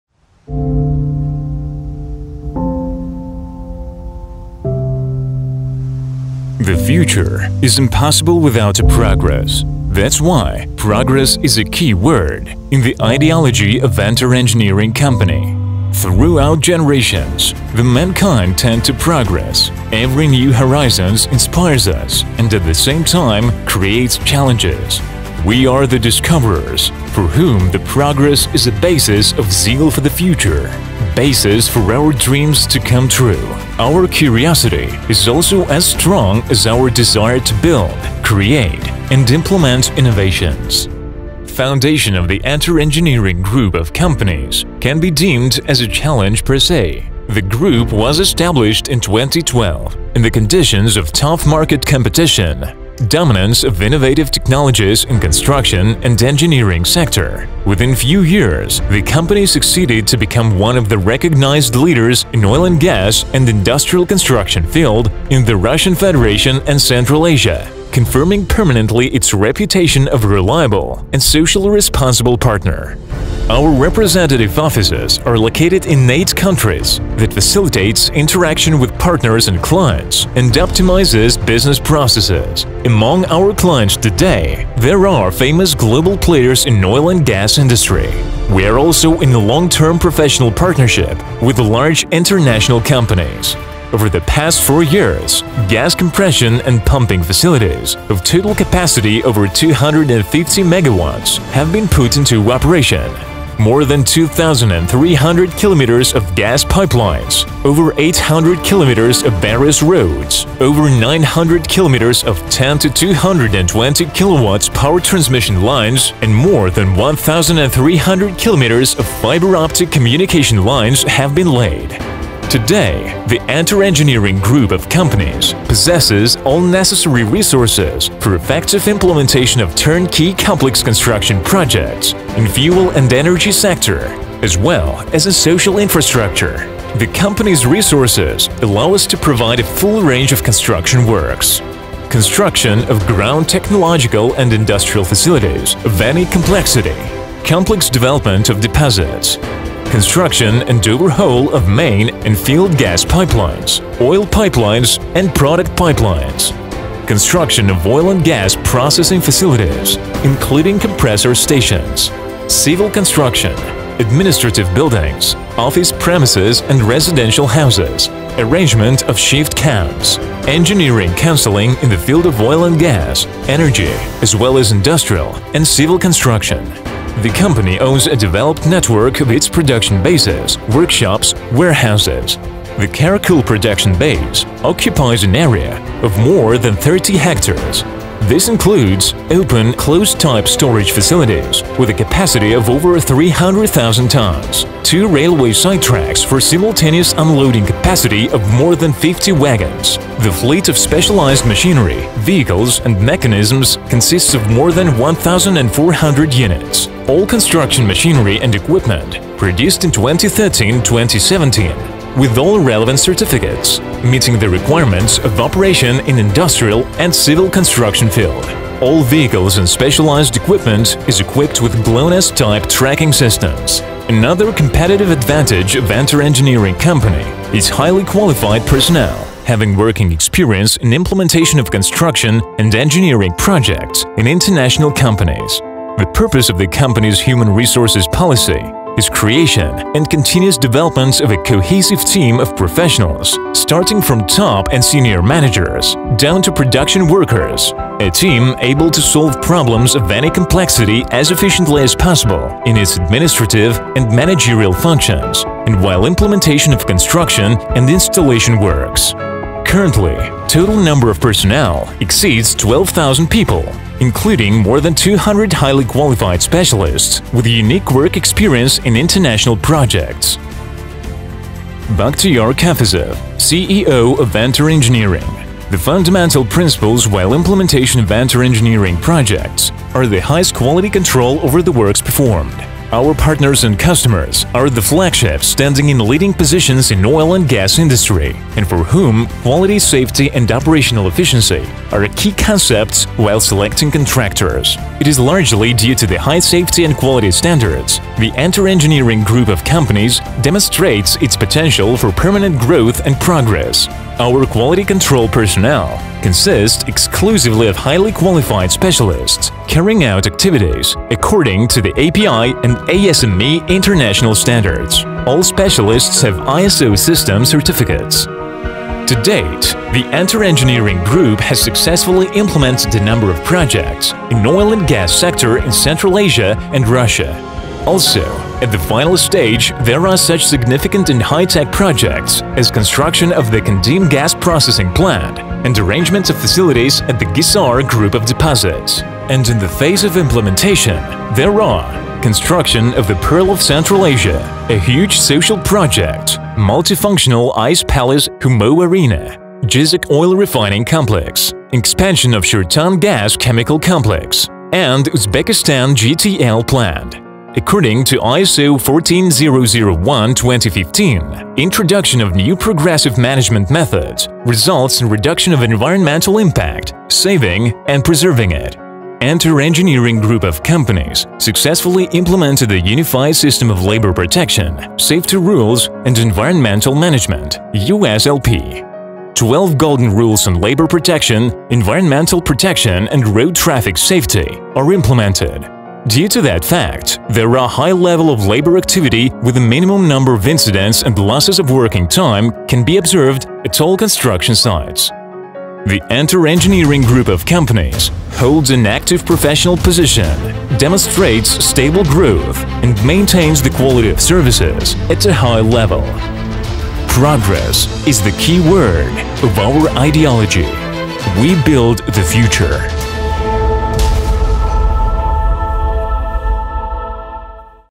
ENTER ENGINEERING UZBEKISTAN
ENTER Engineering Group is specializing in the construction of industrial and technological facilities of any complexity. With its vast experience in oil and gas industry, ENTER Engineering executes EPC contracts and provides a whole range of construction works while implementing “turnkey” projects.

The Group is one of the acclaimed leaders in the field of oil and gas construction in Central Asia and Russian Federation. One of the Group’s shareholders is Gazprombank.

Among its clients are such companies as LLC “LUKOIL Uzbekistan Operating Company”, JSC “Arctic Gaz”, OJSC “Gazprom”, OJSC “Gazprom Zarubezhneftegaz”, JSC “AK Transneft”, JSC “NK Rosneft”, Hyundai Engineering and Construction, Petronas, NHC “Uzbekneftegaz” and others.